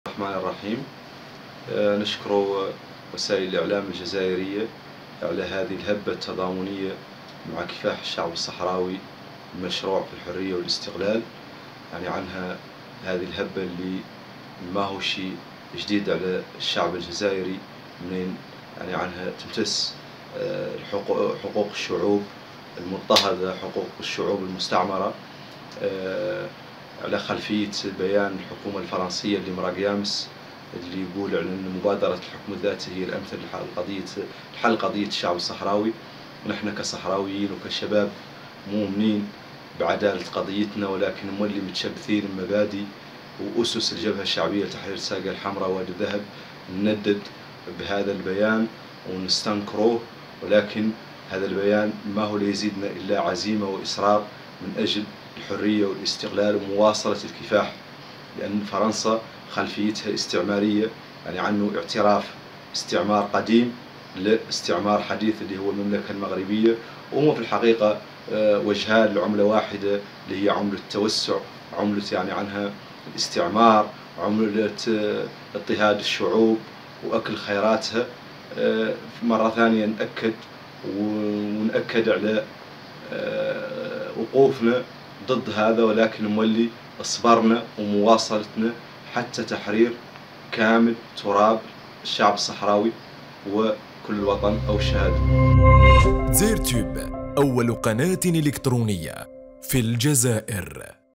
أبهاه الطيب ناشط إعلامي صحراوي يقصف فرنسا ويشيد بالجزائر.."بيان فرنسا يزيدنا عزيمة وإصرار"
شاهد..في تصريح خاص لدزاير توب..أبهاه الطيب ناشط إعلامي صحراوي يقصف فرنسا ويشيد بالجزائر.."بيان فرنسا يزيدنا عزيمة وإصرار"